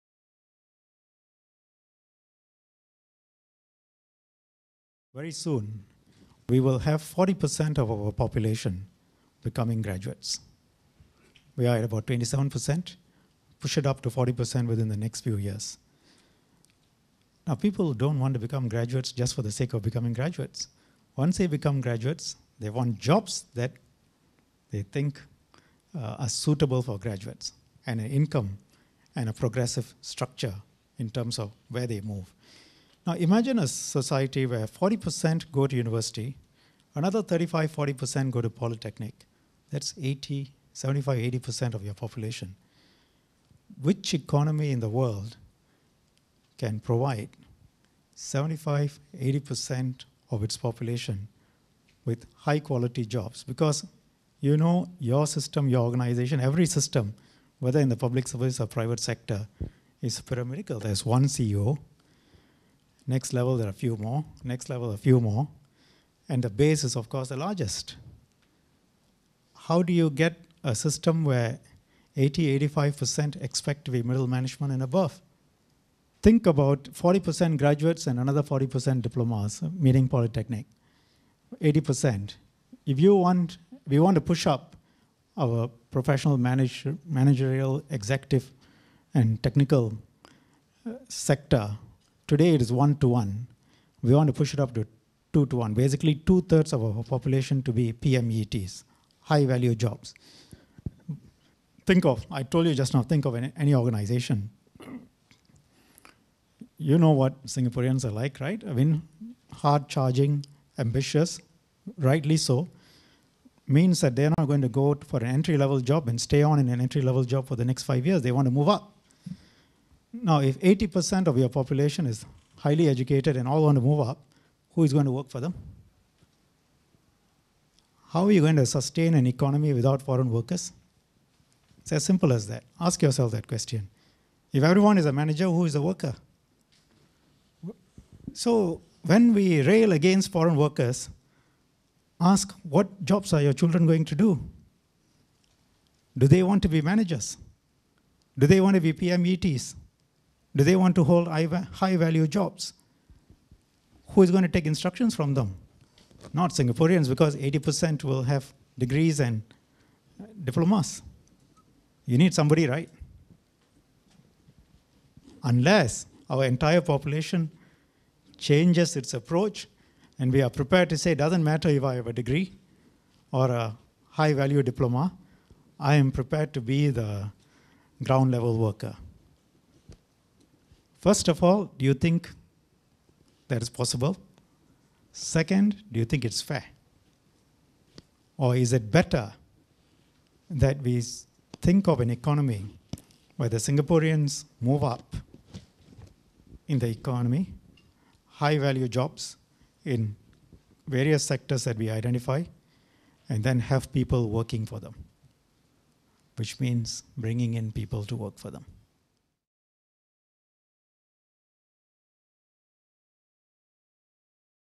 Meeting the expectations of an increasingly educated society: Minister K Shanmugam
At a REACH dialogue session on population issues, Minister for Foreign Affairs and Law, K Shanmugam, explains how the Government is working towards meeting the expectations and aspirations of an increasingly educated society.